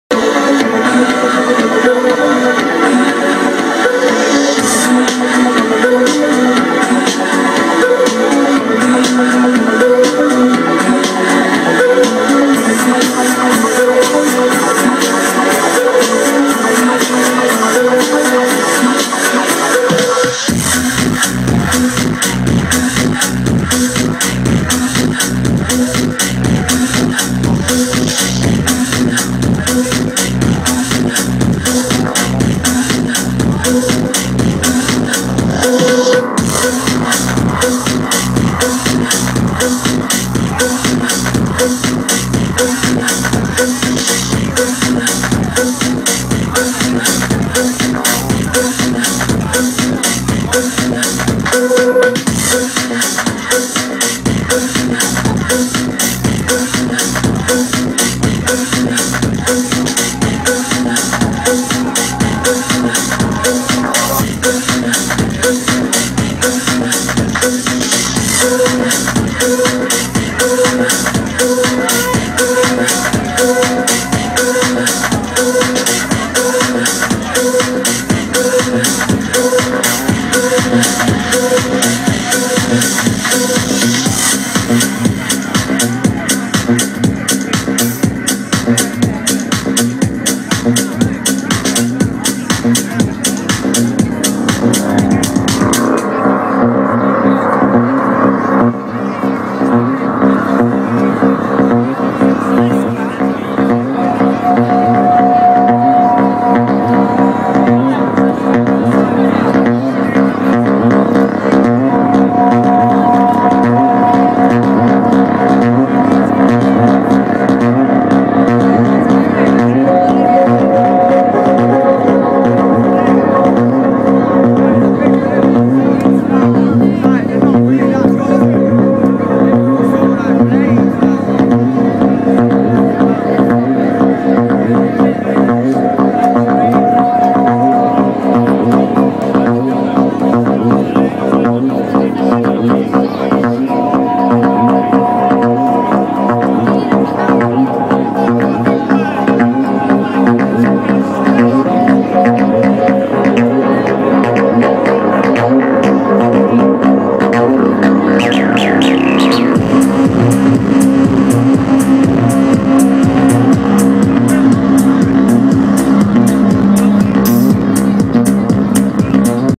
Hardway Bros, Unknown Festival Croatia 2013, Mad Ferret's Mirror World Stage
Tremendous set from Sean Johnston (Hardway Bros) at Unknown Festival on the Mad Ferret Mirror World Stage, Thursday 12th September 2013, Rovinj, Croatia